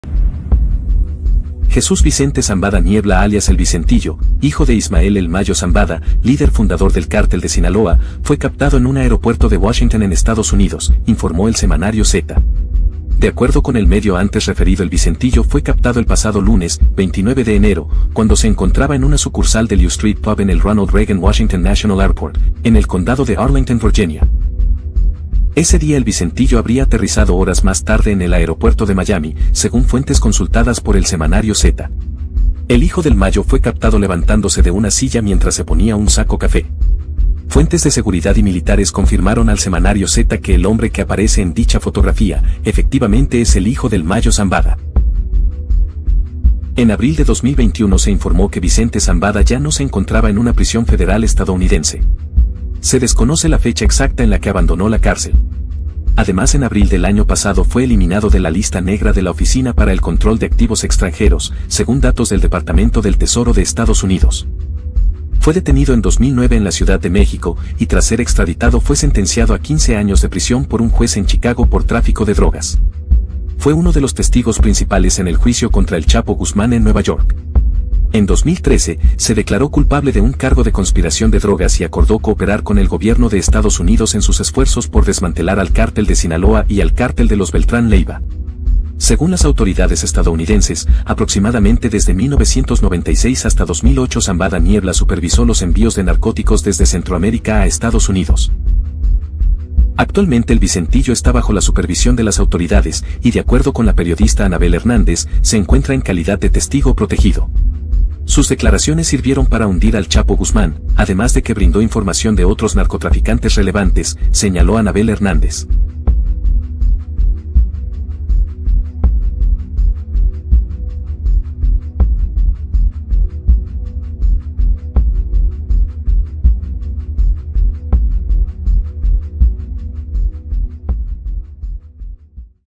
Así se ve ahora "Vicentillo", hijo de "El Mayo" Zambada, fue captado en un aeropuerto de Washington
EL UNIVERSAL entrevistó a Adela Navarro Bello directora del Semanario Zeta quien recibió la fotografía e inició la investigación sobre su estatus en México